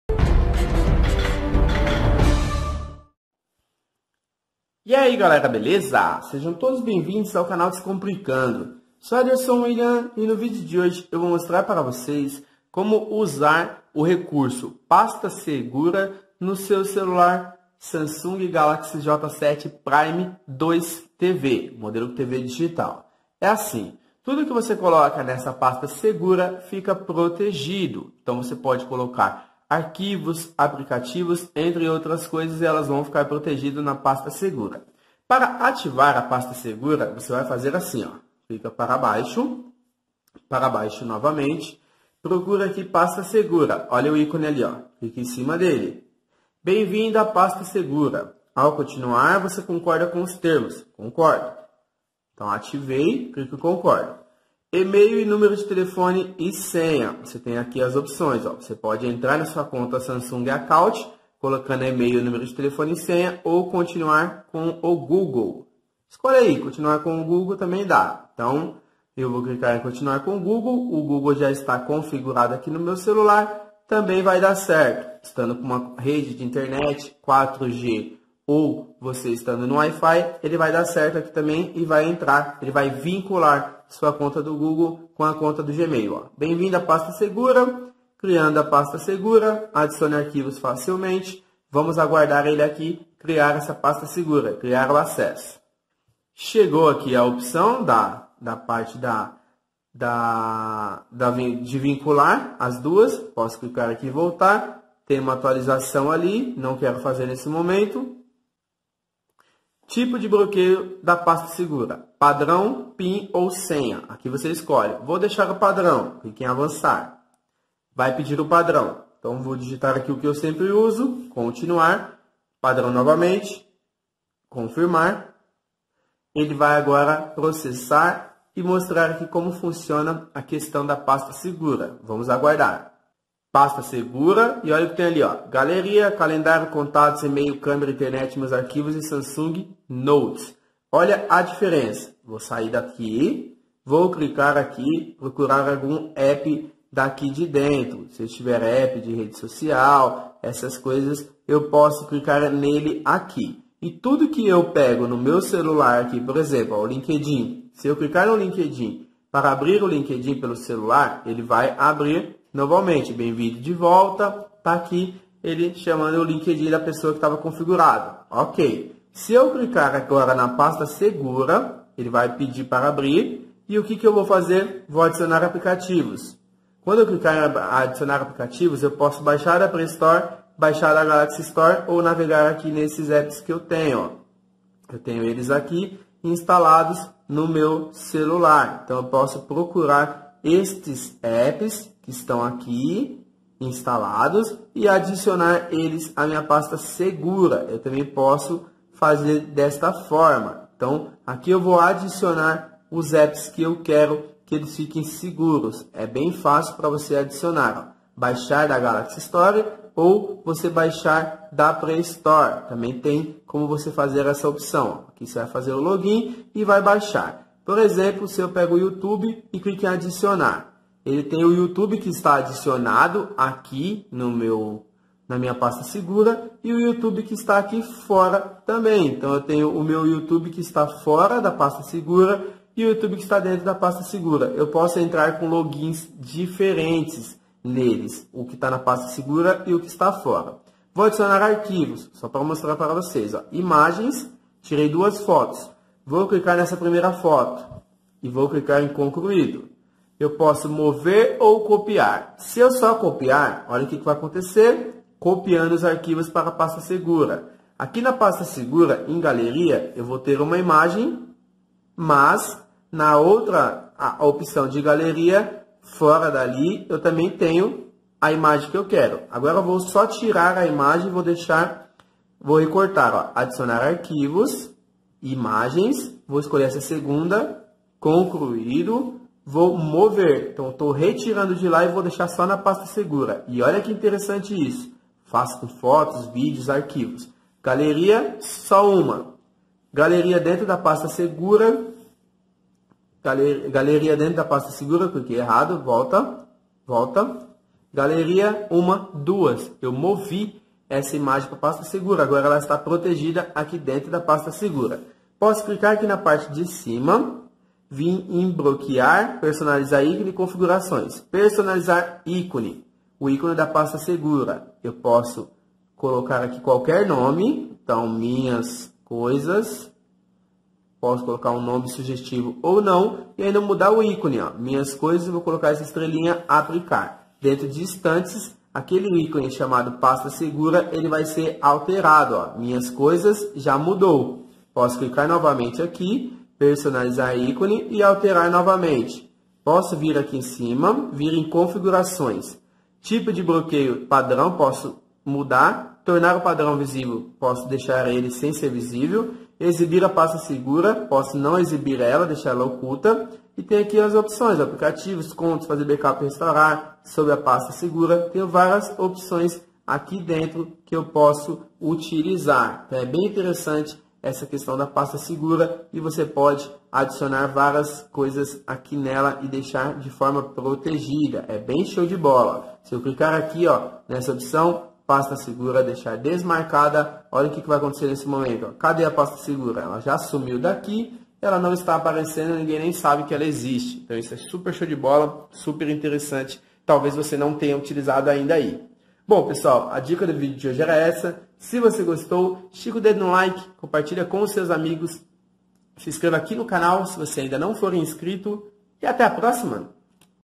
Como Usar a Pasta Segura do Celular Samsung Galaxy J7 Prime2 e Ter Arquivos e Apps Protegidos
Veja neste vídeo como proteger fotos, vídeos, arquivos e aplicativos usando a pasta segura em celulares da Samsung. Você pode usar a função nativa chamada pasta segura e assim deixar o acesso a contas nos Apps protegidas e ainda esconder fotos, vídeos e arquivos no seu Smartphone.